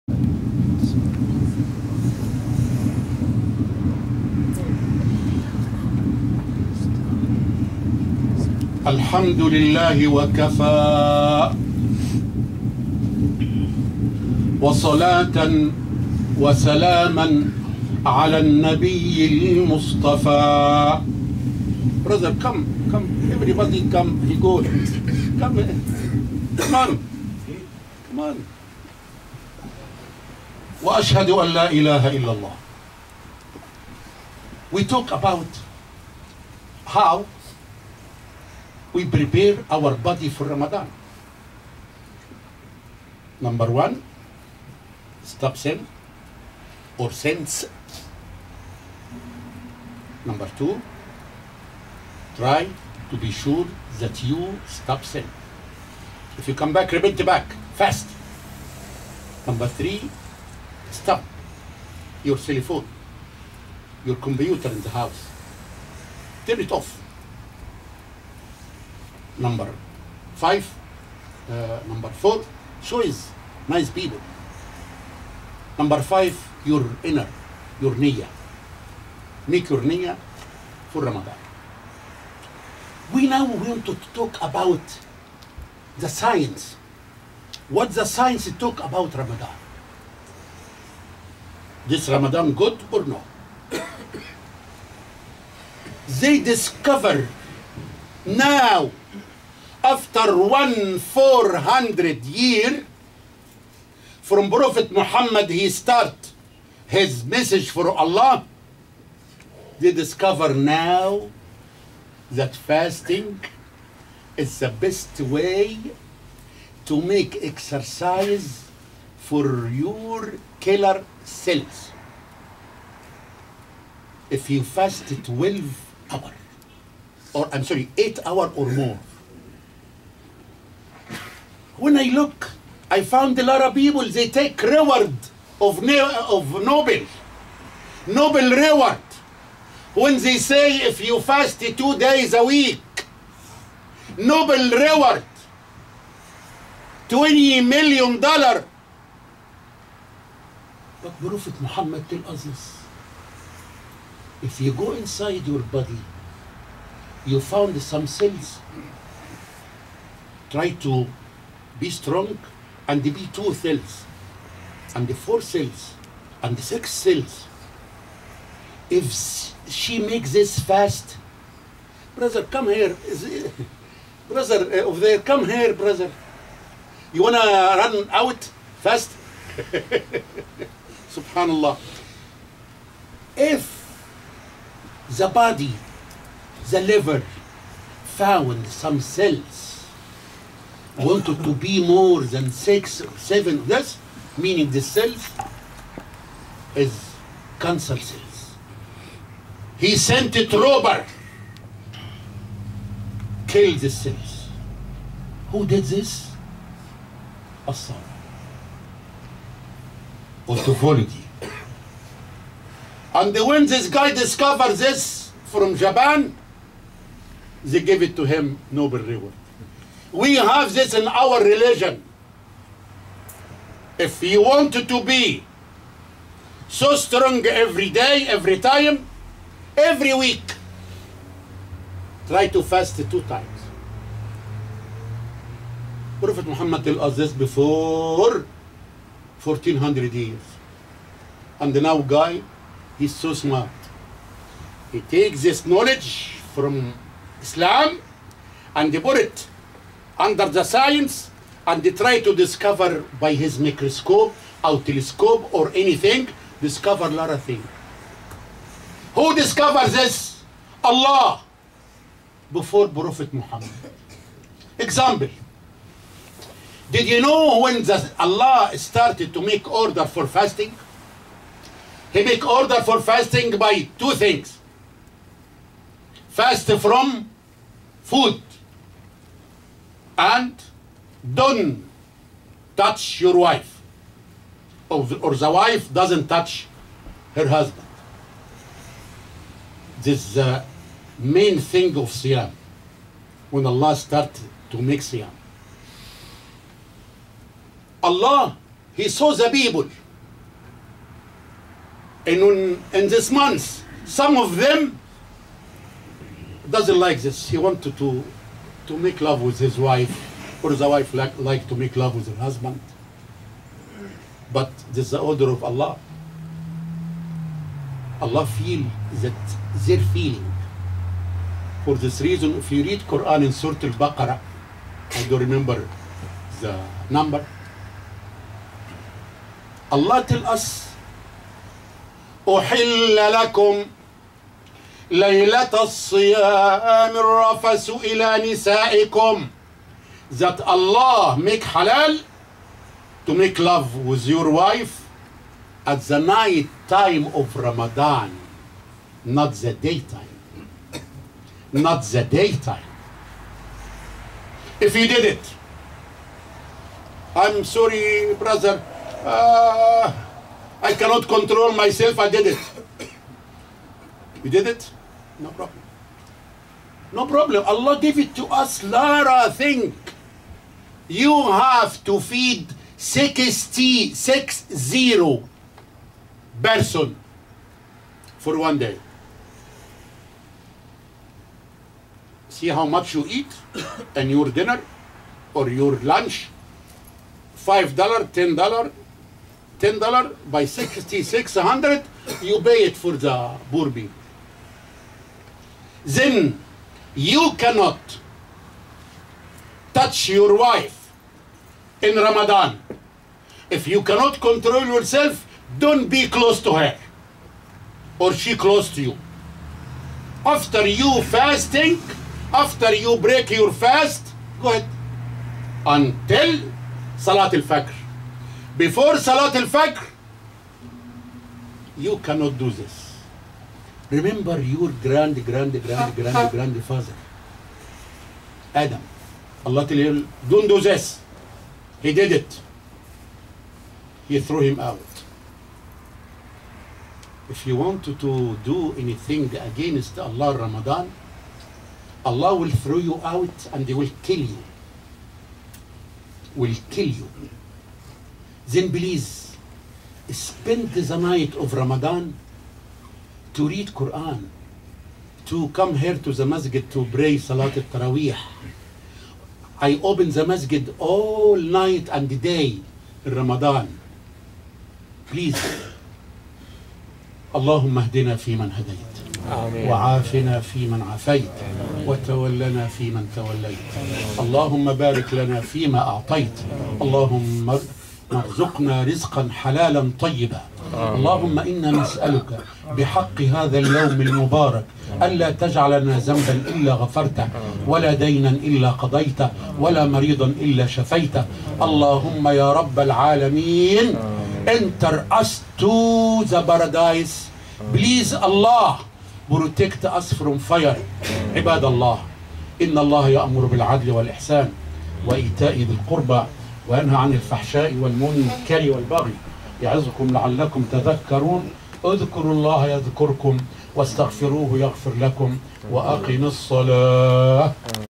sheikh Mustafa Friday lecture (Khutbah) in new jersey masjid 8 mints
Friday lecture by sheikh Mustafa every Friday khutbah short clip